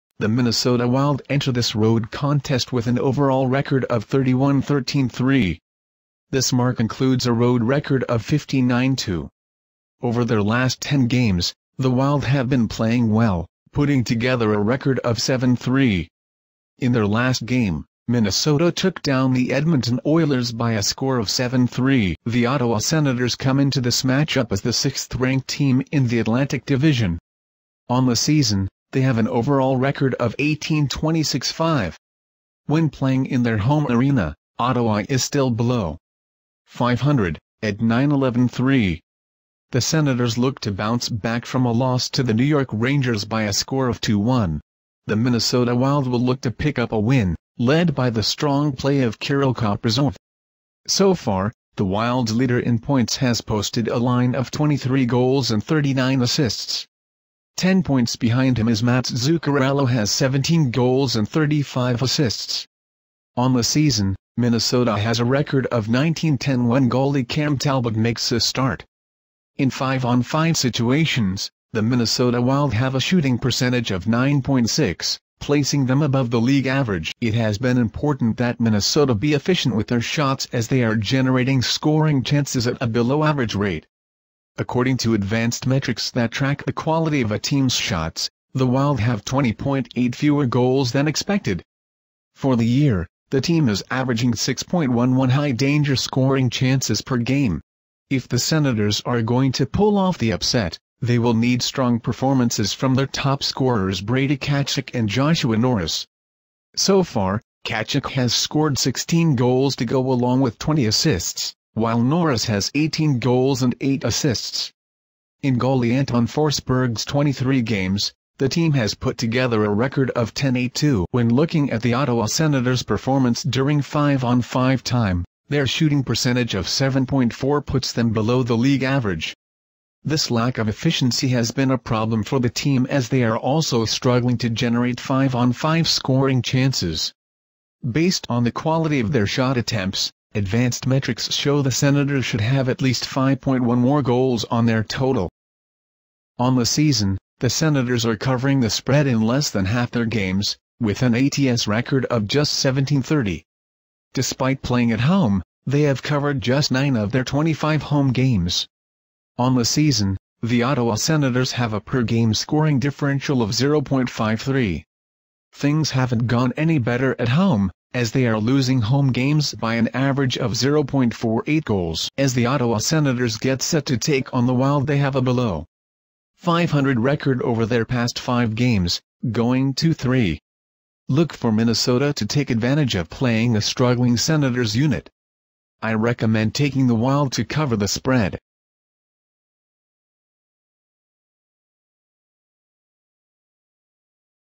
Minnesota -1.5 WIN! Minnesota Wild vs Ottawa Senators, 2/22/22, NHL Pick, Prediction and preview.
Preview and prediction of the matchMinnesota Wild vs Ottawa Senators, 2/22/22, NHL Pick, bets and odds.